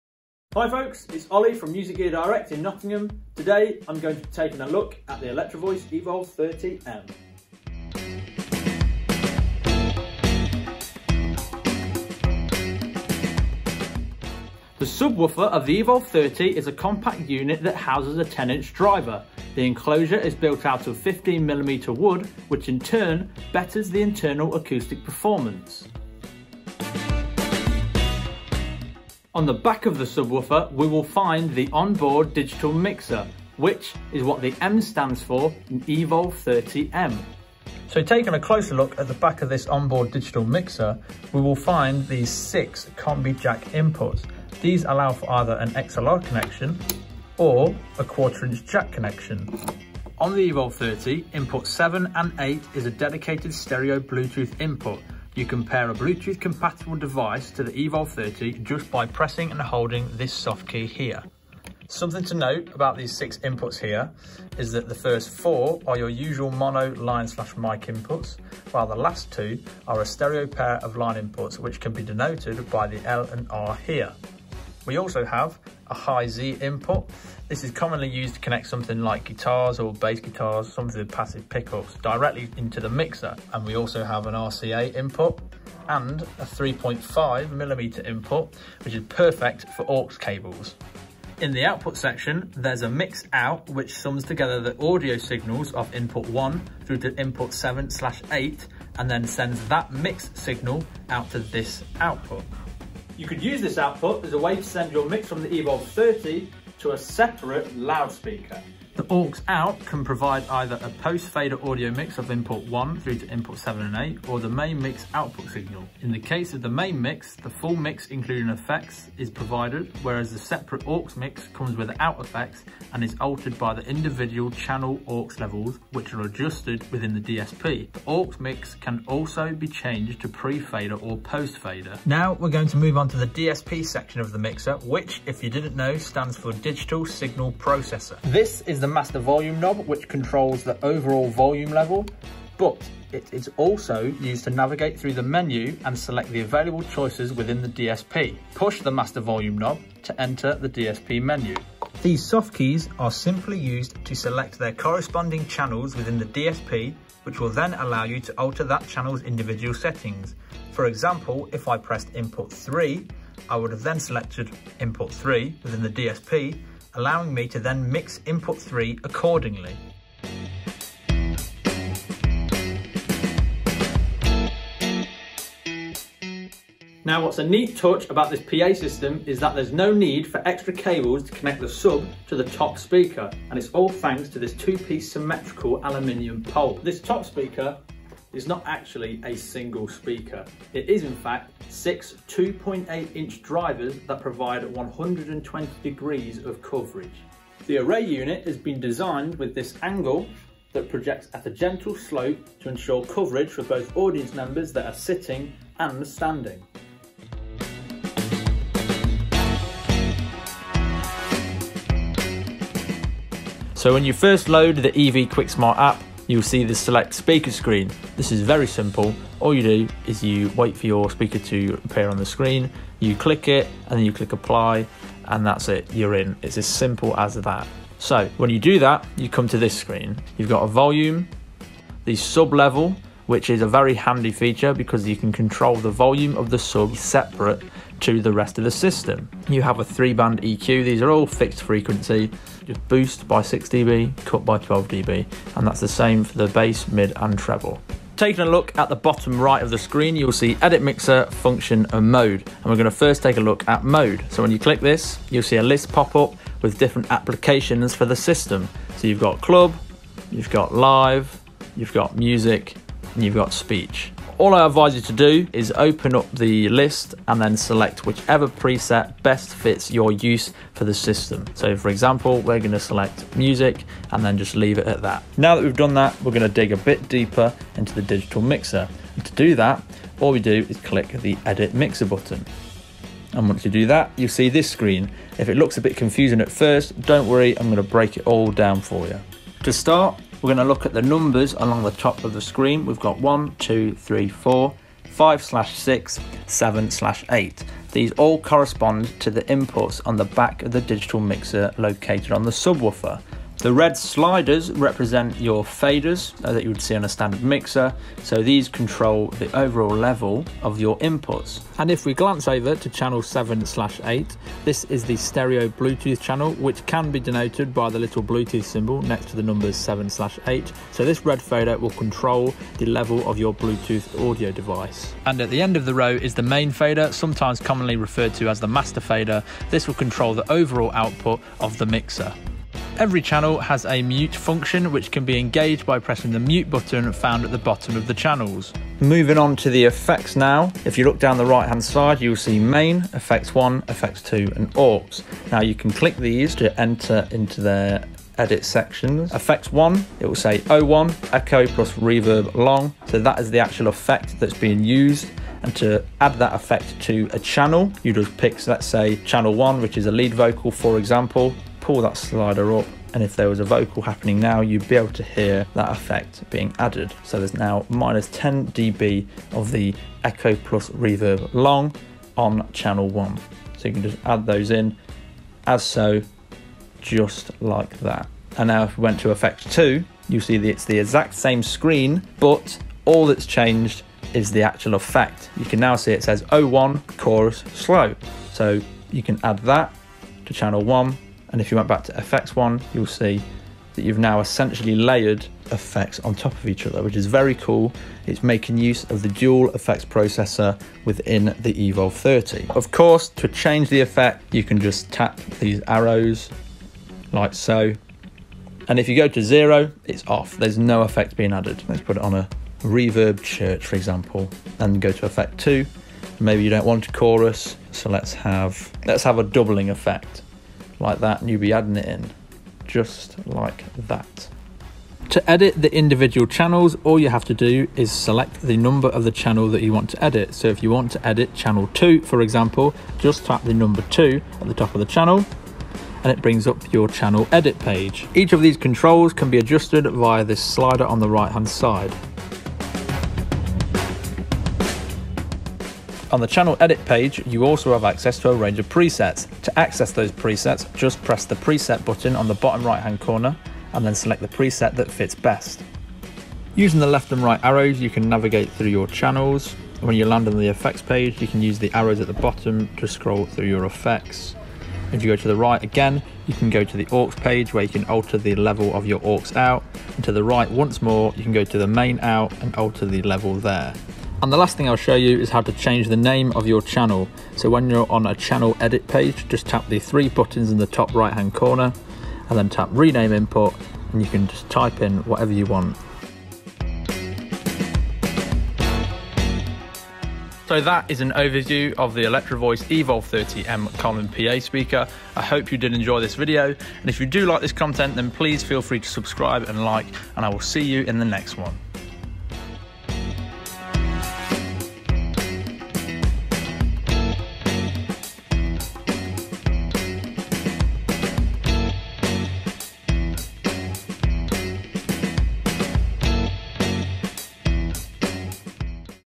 Electro-Voice EVOLVE 30M Active Column PA System | OVERVIEW
Hi folks, in this video we are taking a look at the Electro-Voice EVOLVE 30M Column PA System. Whether you're a solo musician, part of a small group, a mobile entertainer, or you're looking to equip a small venue with a high-performance audio system. The Evolve 30M boasts a whole host of components, options, and tools to give you the sound you deserve. An integrated programmable 8-channel digital mixer, a powerful 1000W Class-D amplifier, intelligent DSP, flexible wireless control, outstanding connectivity... The list goes on. But no matter what (or where) you want to use your Evolve 30M, there's one thing you can count on. This is a system that was built to perform. And from the moment you power it on, you'll understand.

Key Video Points
00:00 - Intro
00:15 - Subwoofer
00:27 - Digital Mixer
02:11 - DSP
02:57 - Top Speaker (Array)
03:30 - EV Quicksmart App
03:48 - Speaker Control Screen
04:35 - Mixer Control Screen
05:39 - Using FX (Effects)
07:05 - How To Use & Edit Channels
07:54 - How To Rename Channels
08:10 - Outro

Get in touch:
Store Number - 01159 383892